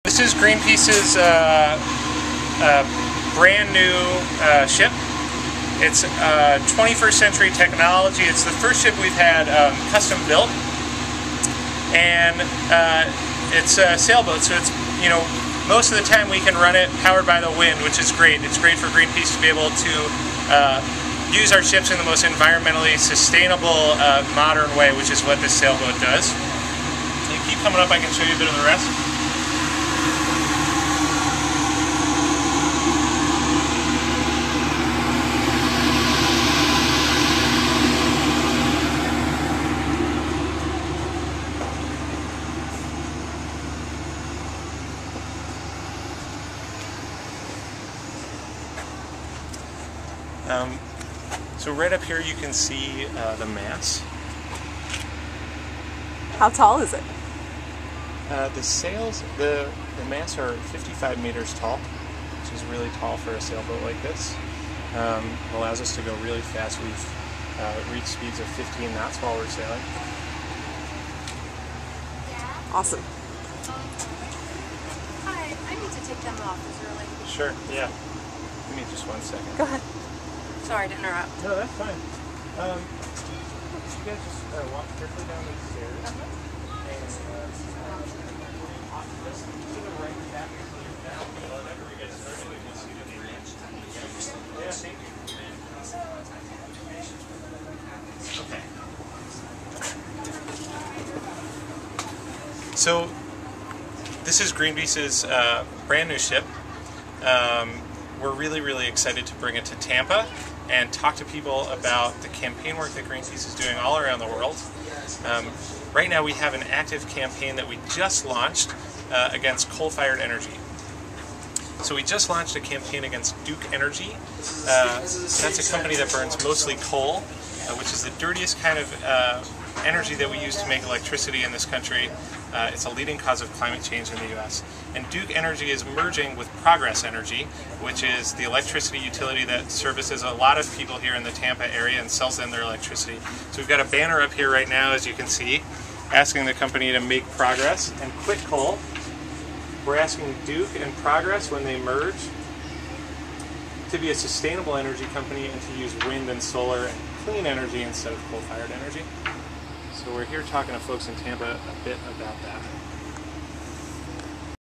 Greenpeace Rainbow Warrior III stops in St. Pete: WMNF News
David Pomerantz guides a tour of the ship and explains how it uses green technology.